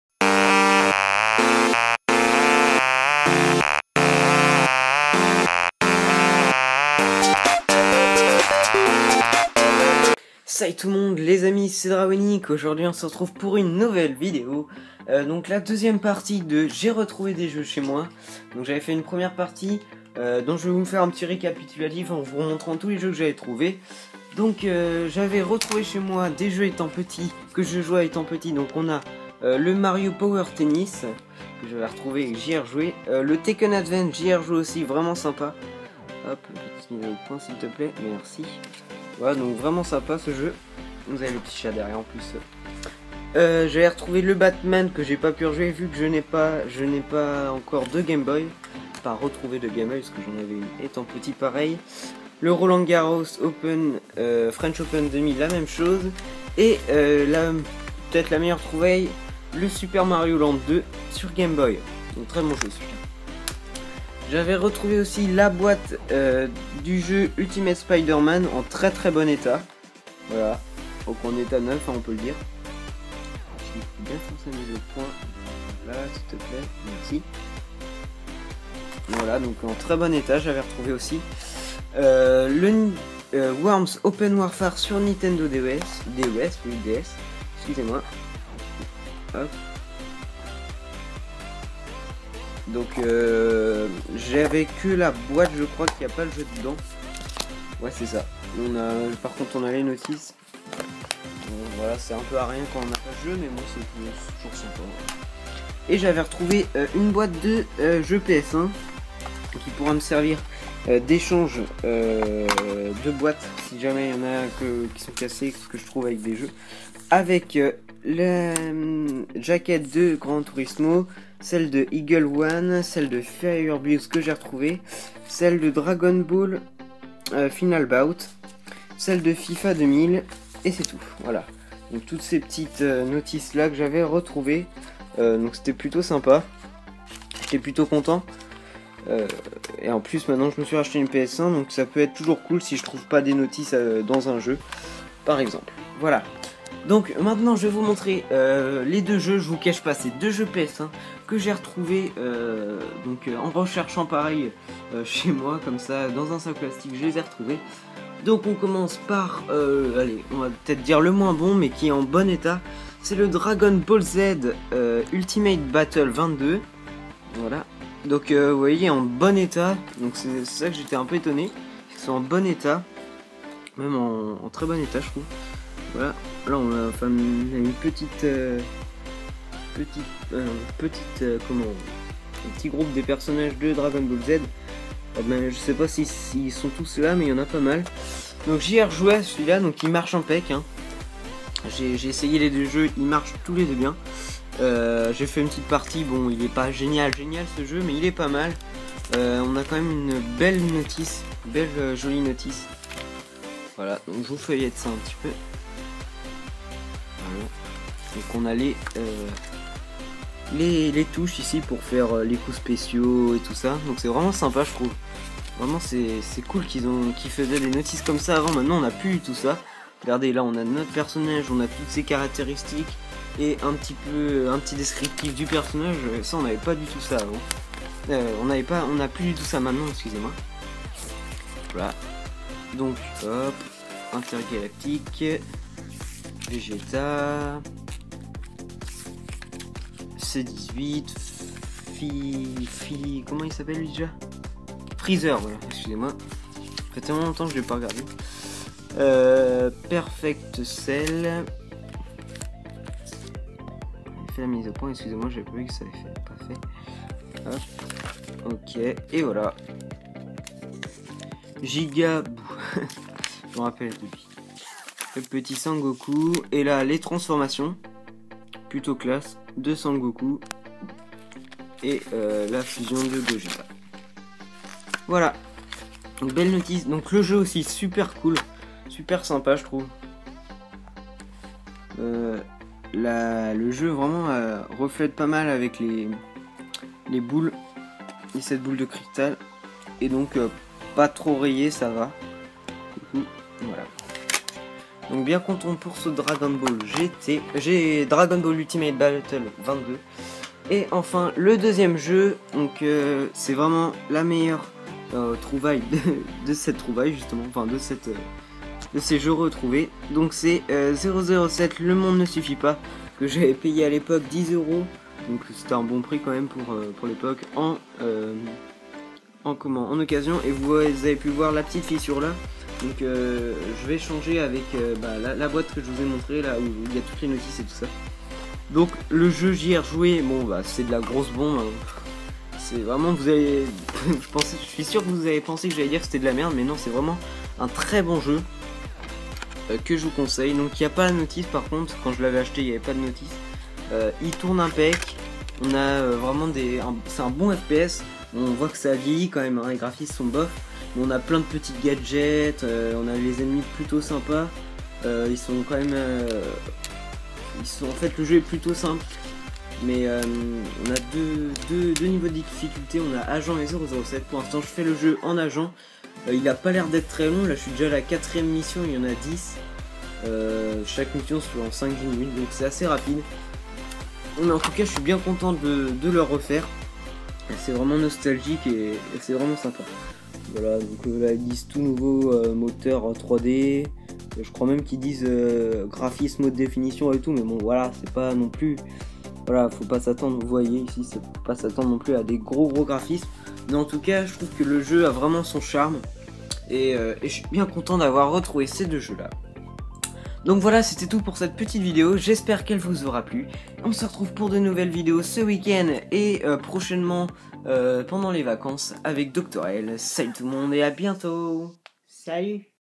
J'ai retrouvé des jeux chez moi #2 (PS1)
Salut tout le monde ! En regardant chez moi et en fouinant un peu partout j'ai retrouvé des anciens jeux auxquelles j'avaient joué étant petit je vous les présentes donc dans cette video !